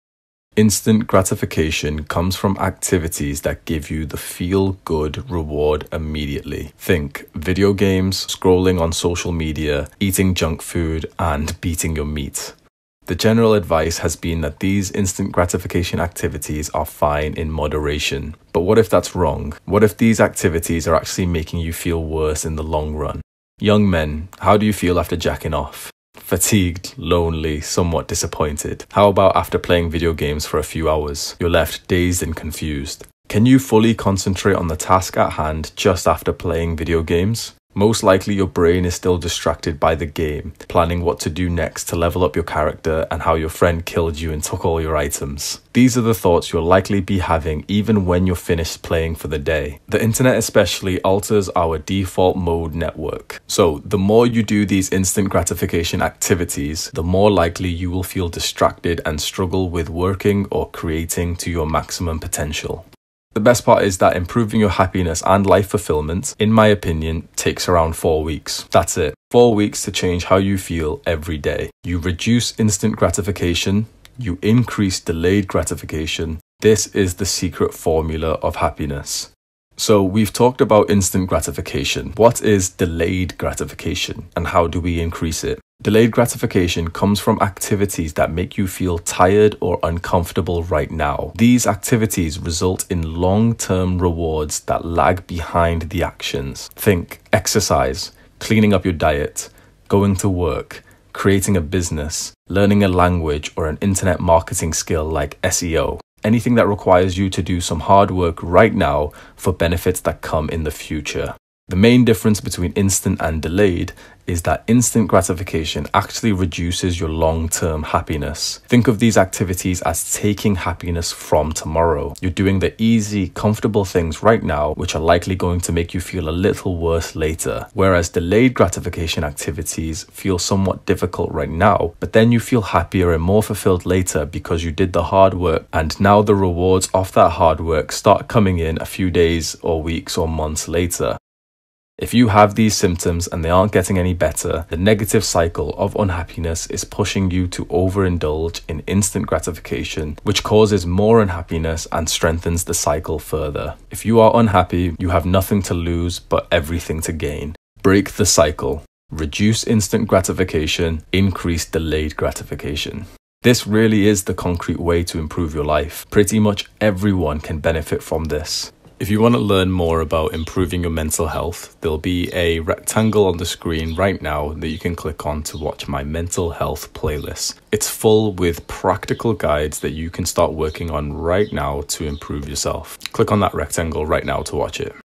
(Reupload from: Hamza) How reducing instant gratification will undoubtedly benefit your future
Just released my 22-page course "Optimizing Testosterone: A Comprehensive Course for Restoring Mental and Physical Health" check it out Reupload in honor of Hamza's content if his channel gets deleted

Instant gratification activities slowly decrease your long term happiness and fulfilment, with just a small boost of instant pleasure whilst you are partaking. Replace this with delayed gratification activities, which slowly INCREASE your long term happiness and fulfilment, at the cost of hard work and discomfort right now.

A good, happy, fulfilled life is one that has a balance between instant and delayed gratification.